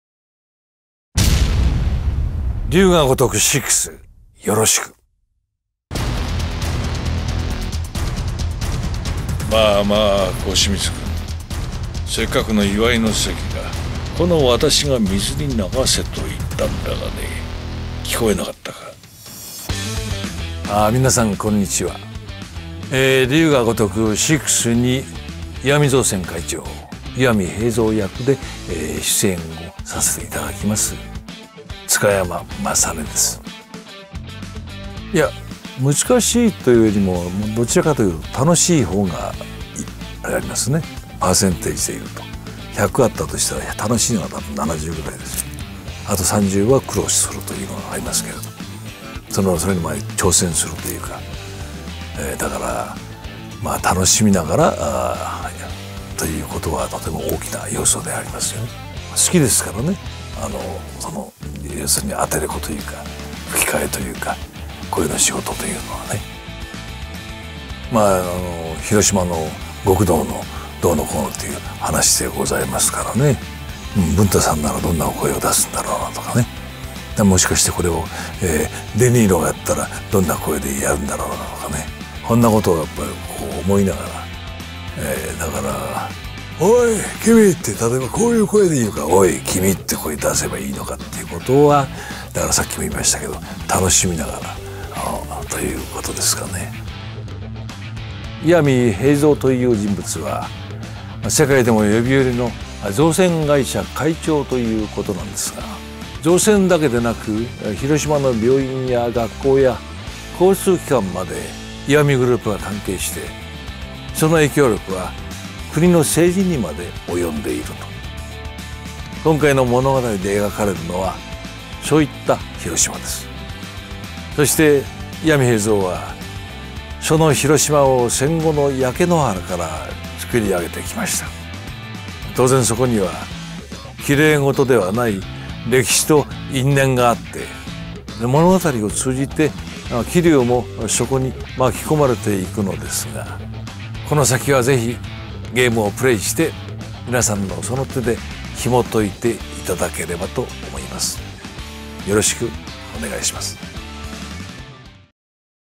PS4専用ソフト『龍が如く６ 命の詩。』津嘉山正種スペシャルインタビュー
PS4専用ソフト『龍が如く６　命の詩。』津嘉山正種(巌見兵三役)スペシャルインタビューを公開しました！

タイトル：龍が如く６　命の詩。
プラットフォーム：PlayStation®4
発売日：2016年12月8日(木)
価格：8,190円(税別)